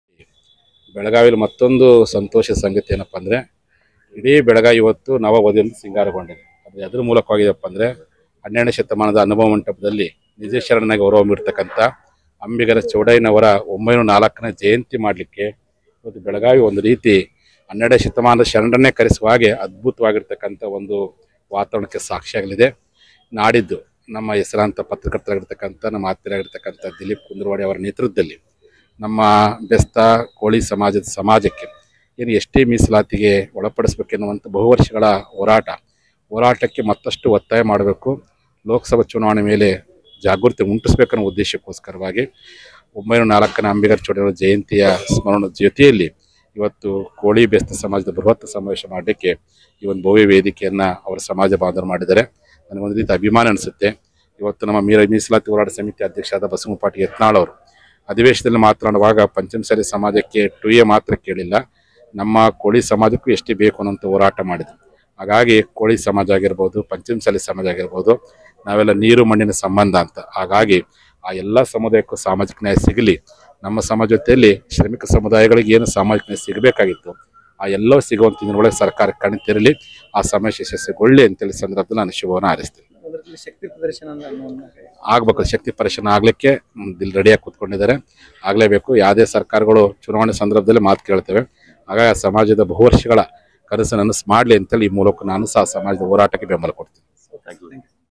ಅಂಬಿಗರ ಚೌಡಯ್ಯ ಜಯಂತಿ ಸಿದ್ಧತೆ
ಅಂಬಿಗರ ಚೌಡಯ್ಯ ಜಯಂತಿ ಆಚರಣೆ ಹಿನ್ನೆಲೆಯಲ್ಲಿ ಪಂಚಮಸಾಲಿ ಪೀಠದ ಜಯ ಮೃತ್ಯುಂಜಯ ಸ್ವಾಮಿಗಳು ವೇದಿಕೆಯ ಸಿದ್ಧತೆಗಳನ್ನು ಪರಿಶೀಲಿಸಿದರು